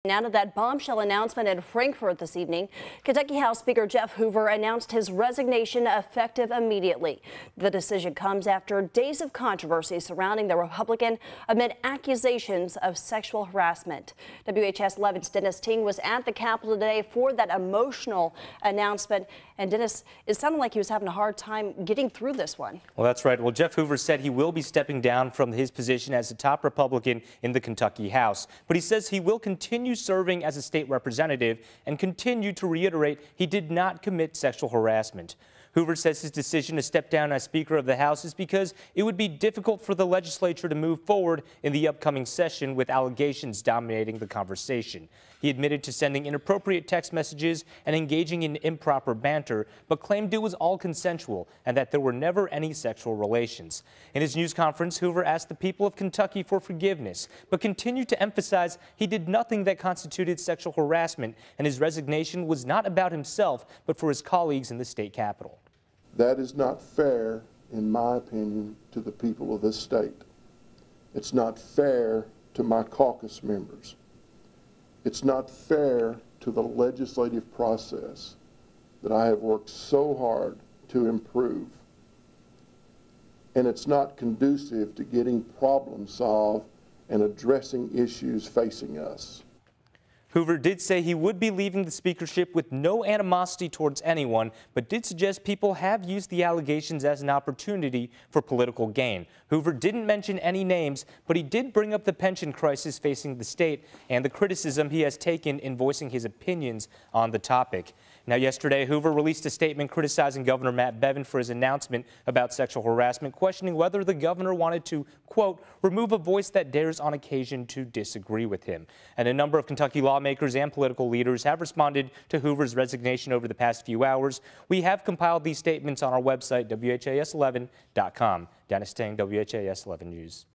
Rep. Hoover steps aside as Ky House speaker following harassment settlement
Kentucky House Speaker Jeff Hoover announced his resignation -- effective immediately.
The decision comes after days of controversy surrounding the Republican amid accusations of sexual harassment.